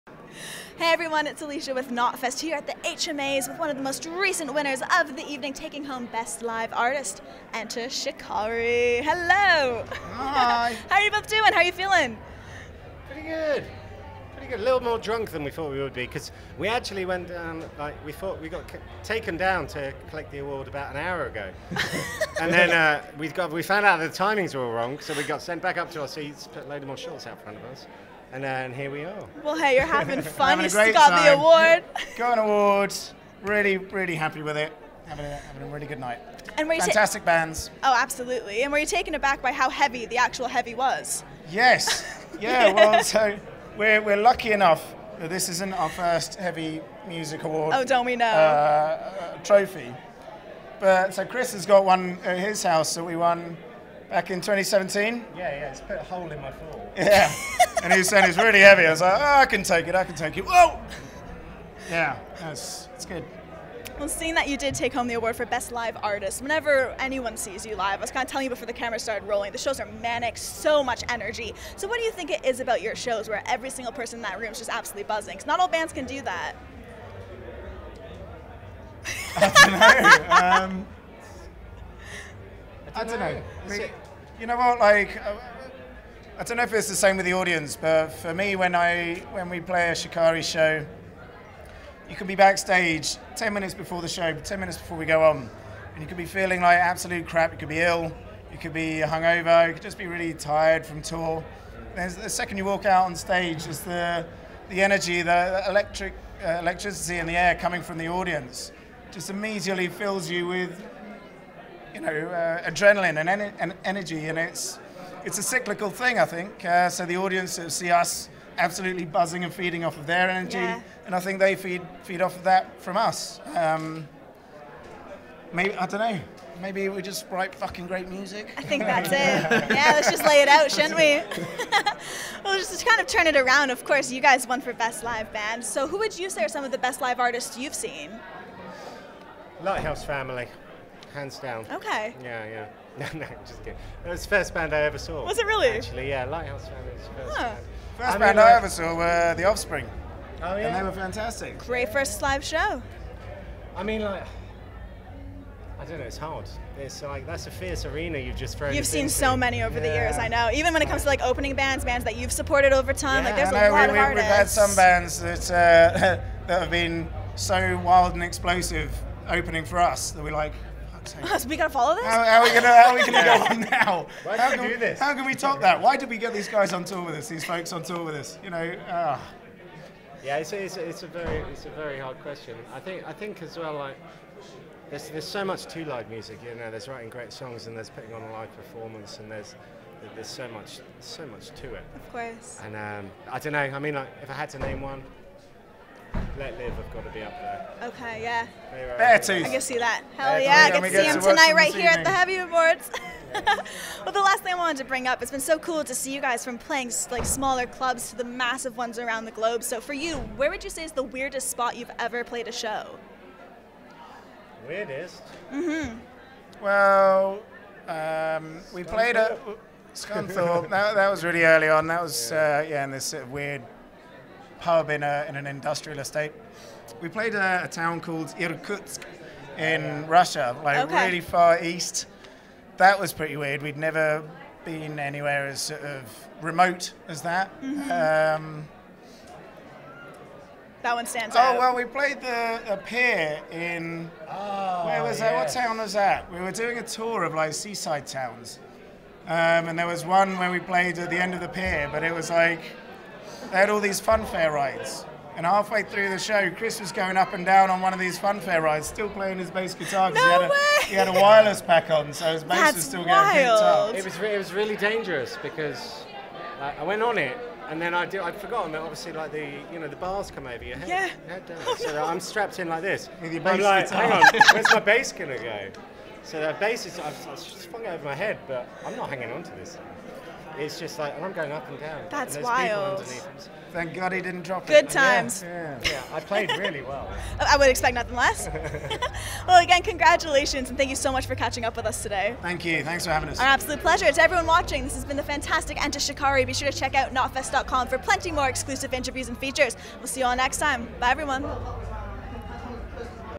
Enter Shikari: Their Most Memorable Show, First & Best Bands They've Seen & more
After accepting the award for Best Live Artist at the Heavy Music Awards, Enter Shikari chats with Alicia Atout on what makes their live shows so special, the first & best bands they've seen live and their most memorable show that included a dangerous fun fair ride.

featuring performances from Beartooth, Holding Absence, Meet Me at the Altar & Static Dress.

The Heavy Music Awards is presented by Amazon Music UK